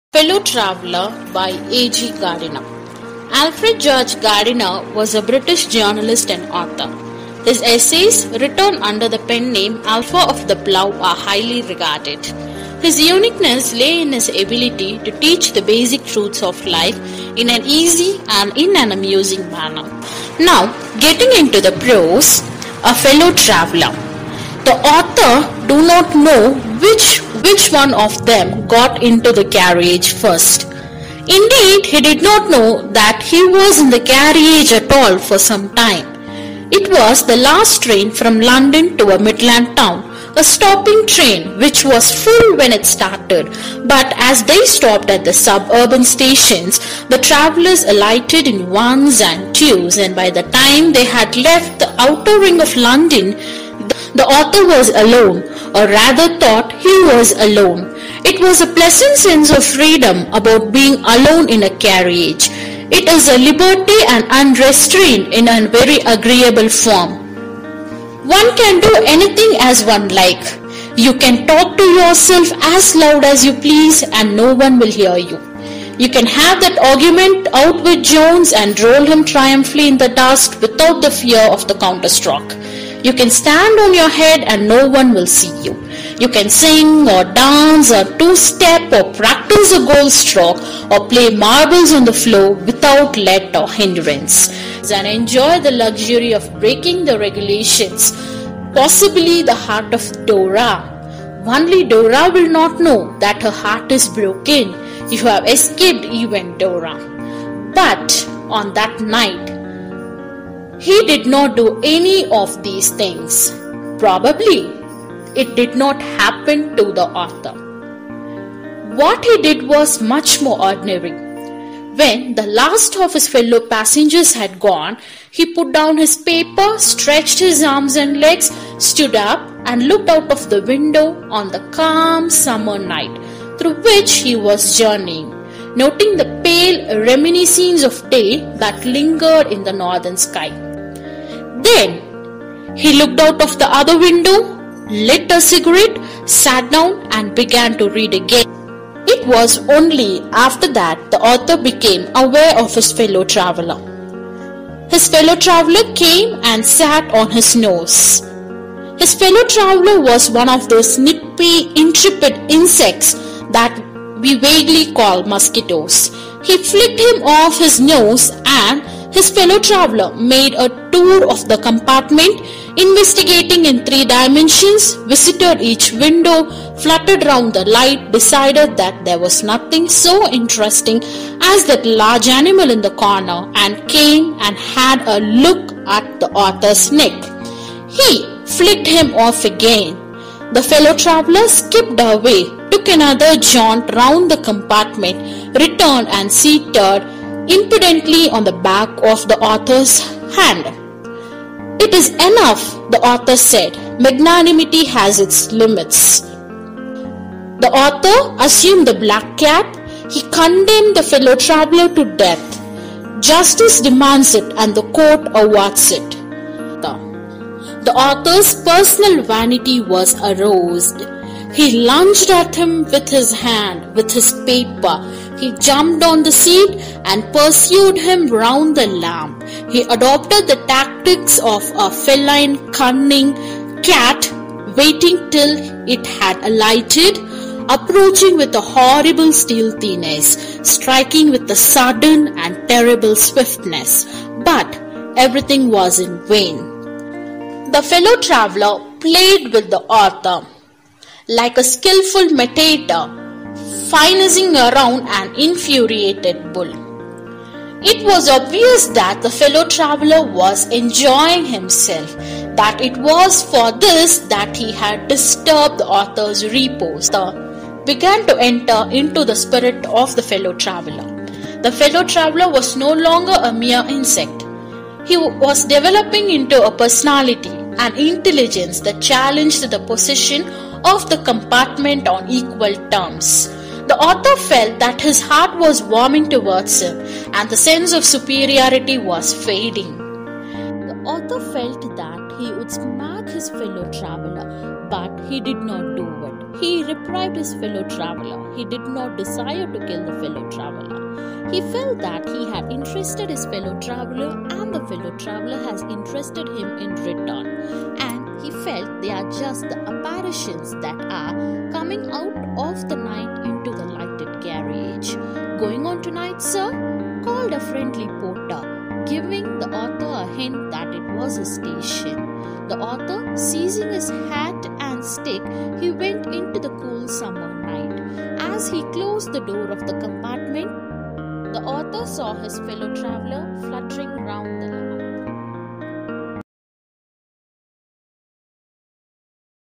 A Fellow Traveller by A.G Gardiner
A short summary on the essay by Professor Mahalakshmi Devan